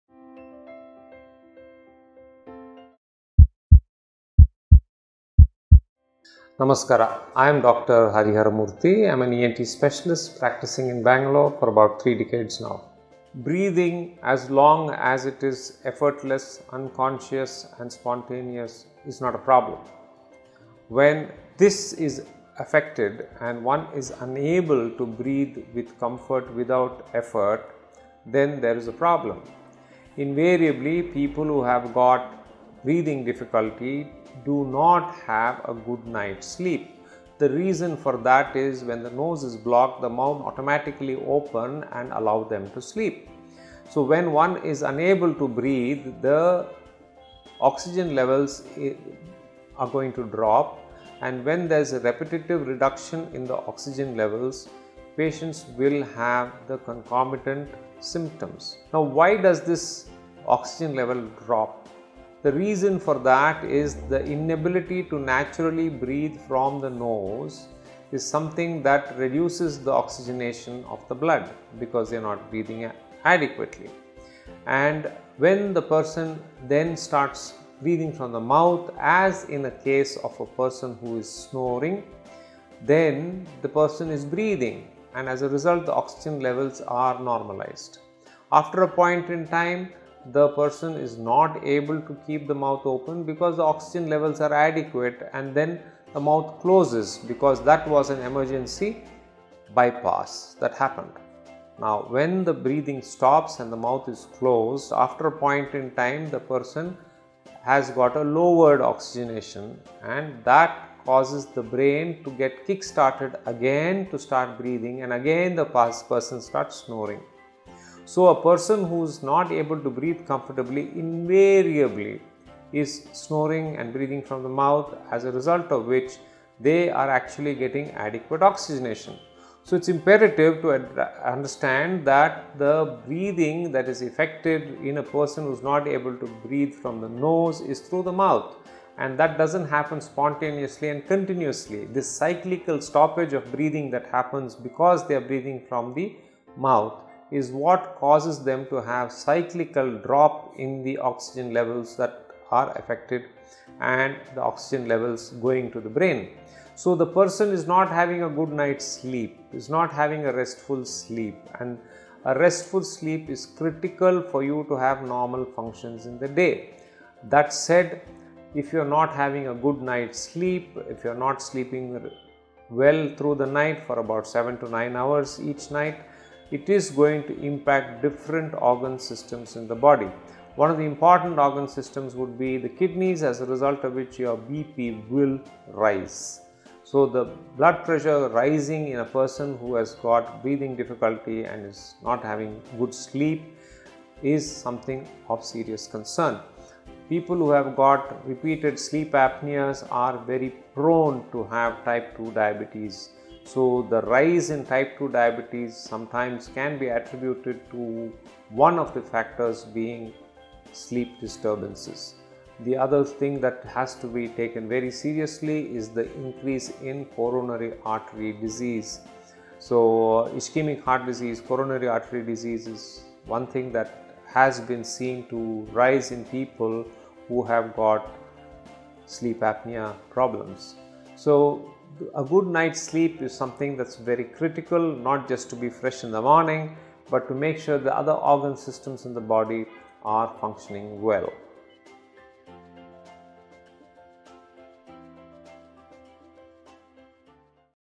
Can Breathing issues affect your Sleep? | Complications - Dr. Harihara Murthy | Doctors' Circle
Dr. Harihara Murthy | Appointment booking no:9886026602
Consultant ENT Surgeon | Murthy Health and Research Center,Bangalore
Breathing  as  long  as  it is  effortless, unconscious and  spontaneous, is  not a  problems, where  this is  affected  and one  is unable to breathe  without  effort  then there is a problem. Invariably people who have breathing  difficulty do not have a good  night sleep.  So when the nose  is blocked the mouth  automatically  opens and allows  them to sleep  so when is  unable to breathe, the oxygen  levels are  going to drop and when there are  drop in the oxygen levels  are  going to drop, the patient is  going to have  concomitant symptoms. Why does  this  oxygen level drop, the reasons is the inability  to  naturally  breathe  from the  nose  is something that  reduces  the  oxygenation of  the  blood  because  they are  not breathing adequately and  when  a person is  breathing from the mouth  as  in a  person who is snoring , and as a result the oxygen  levels are  normalized. People who have rise in sleep  apnea have  Type 1 diabetes.